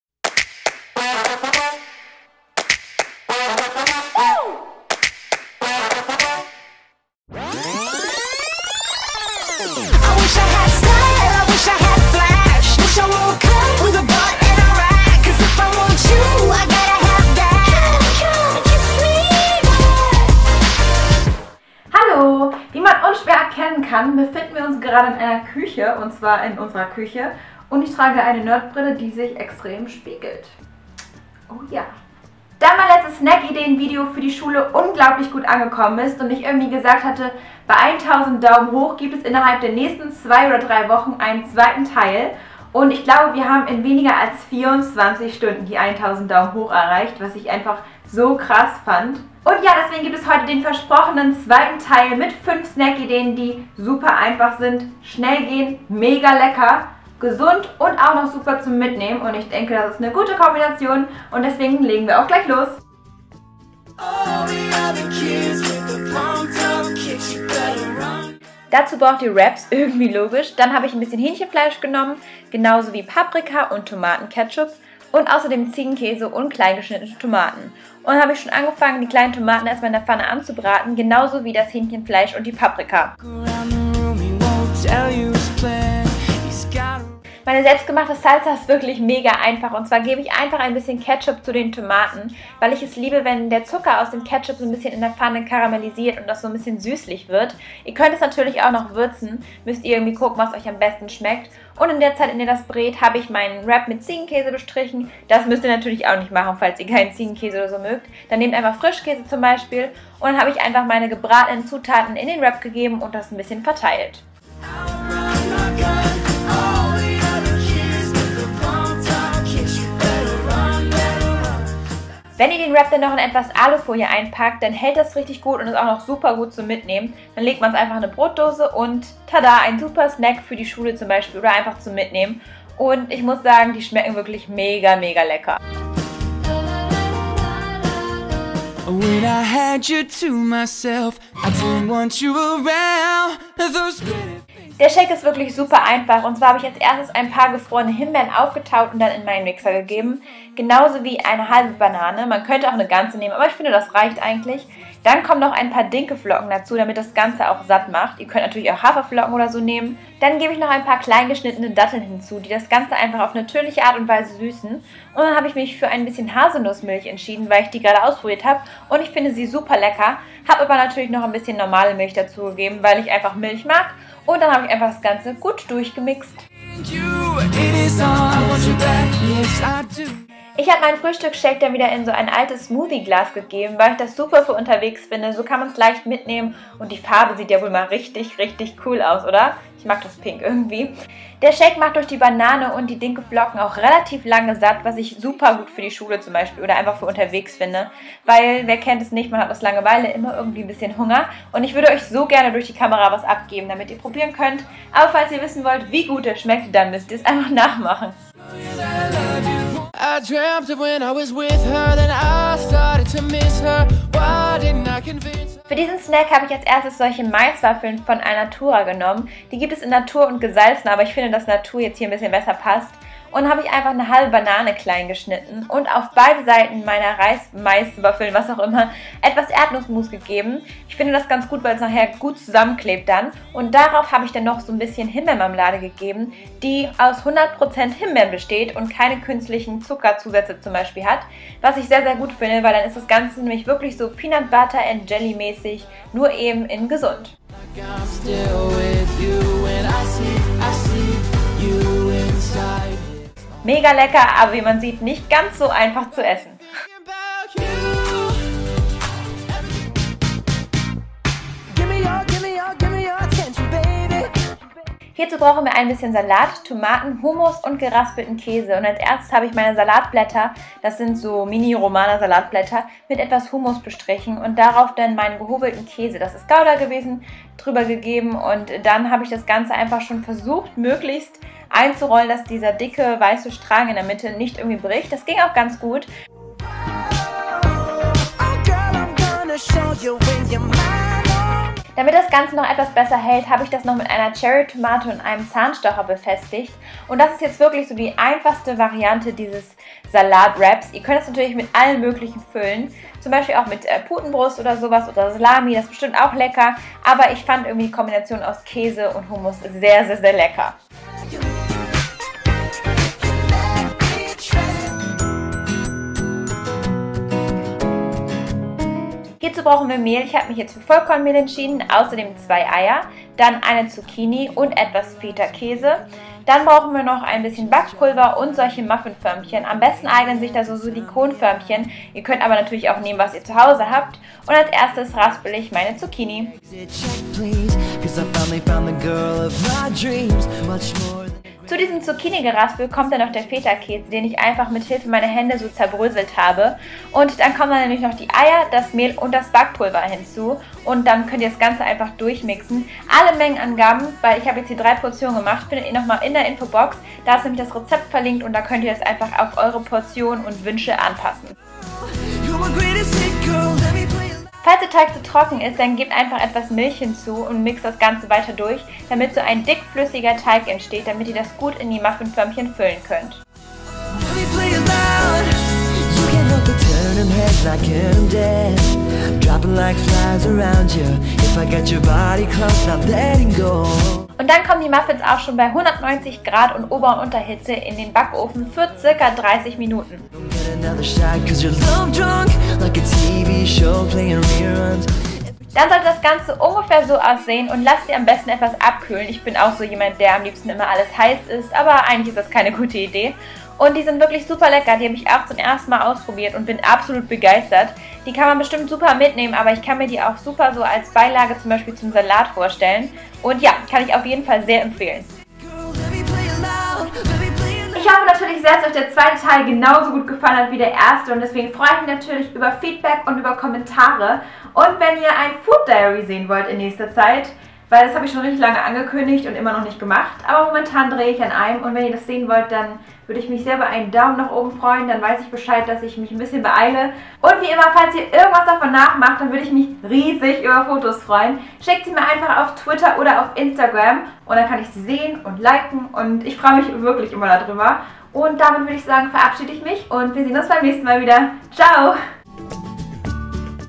5 GESUNDE & SCHNELLE SNACK-IDEEN #2 für die SCHULE & UNTERWEGS!
Hey! Heute zeige ich euch 5 schnelle, einfache, leckere & gesunde Snack Ideen, die perfekt sind für Schule & unterwegs!

Ich hoffe sehr, dass euch das Video gefällt freue mich auf euer Feedback.

SCHON GESEHEN?

Giulia

  Alle Produkte habe ich selbst gekauft und alle Meinungen sind
  meine eigenen.